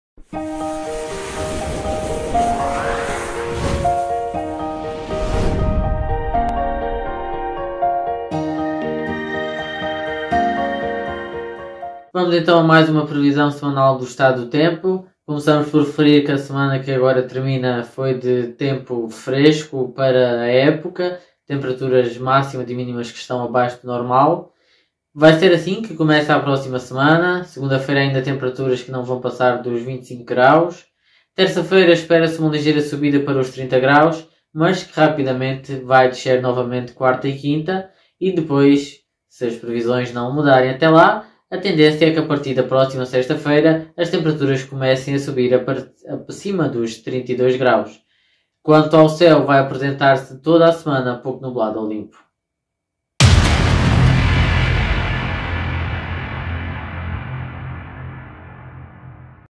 METEOROLOGIA COM MeteoAlentejo
Previsão meteorológica para os próximos dias, com a colaboração da MeteoAlentejo.

PRODUÇÃO:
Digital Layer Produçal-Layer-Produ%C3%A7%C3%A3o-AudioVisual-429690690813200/